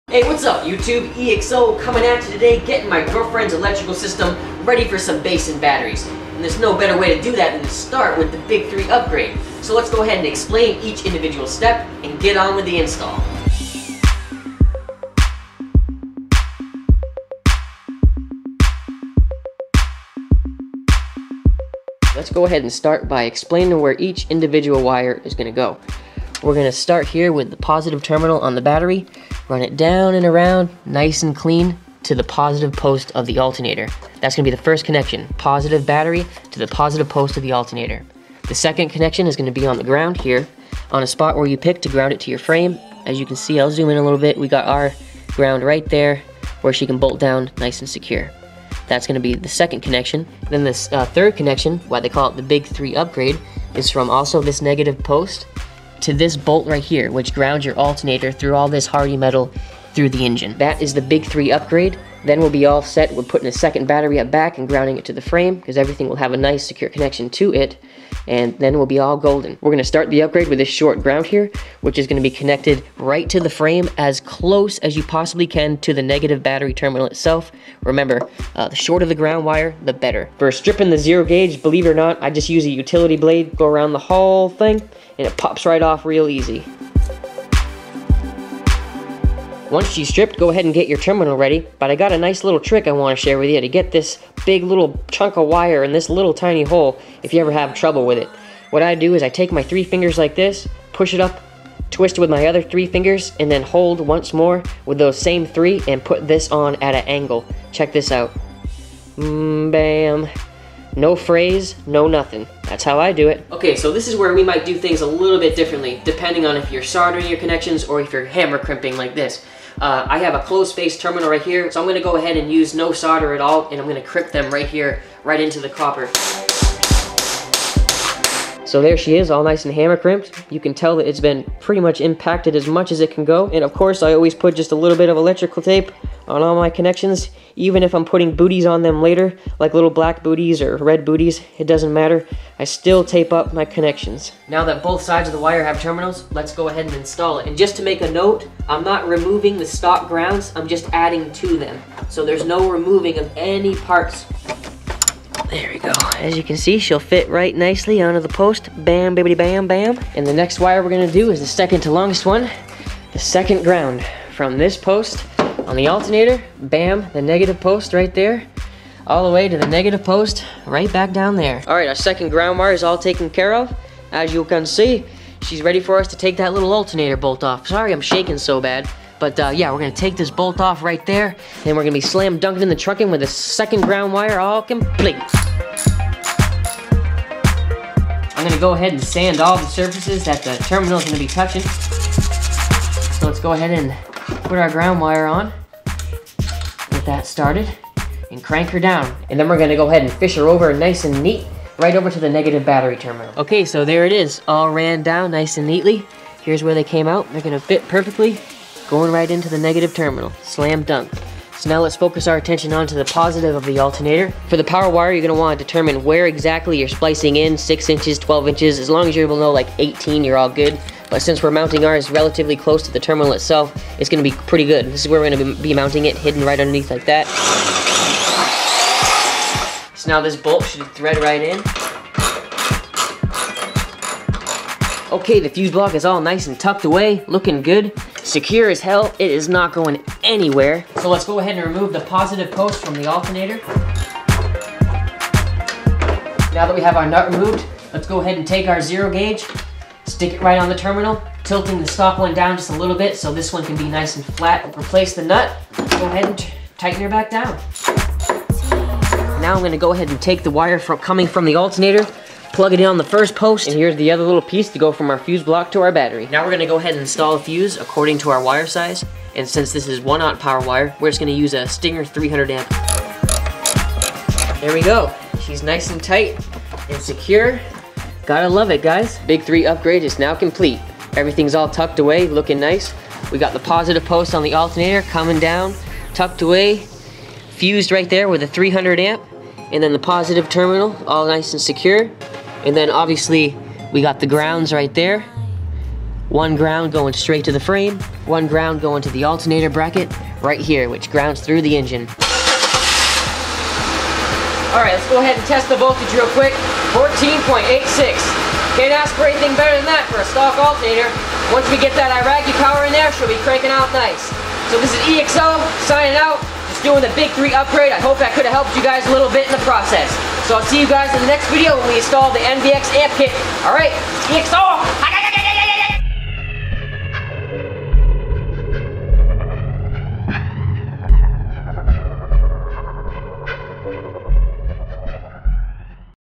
How To Install BIG THREE Upgrade | EXO's BIG 3 Car Audio Wiring Tutorial to Increase Power Flow
Preparing the Impala for a second battery and bass amps! Most stock electrical systems need a little beefing up, so let's get down to it and do the BIG THREE UPGRADE!

Tools Needed:
1/0 AWG Power & Ground Wire
1/0 Ring Terminals
Hammer Crimp or Solder & Torch
Wire Cutters
Utility Knife
Socket Set
1/0 Fuse Holder
300A Fuse

Want to know what to do? Here ya go!
Step 1: Upgrade Battery Ground to Frame
Step 2: Upgrade Battery Ground to Engine Black / Alternator Case
Step 3: Upgrade Battery Positive to Alternators Positive Post

Simple as that! NOW YOU'RE DONE!

2004 Chevy Impala
EXOcontralto
Bassheads Unite 2014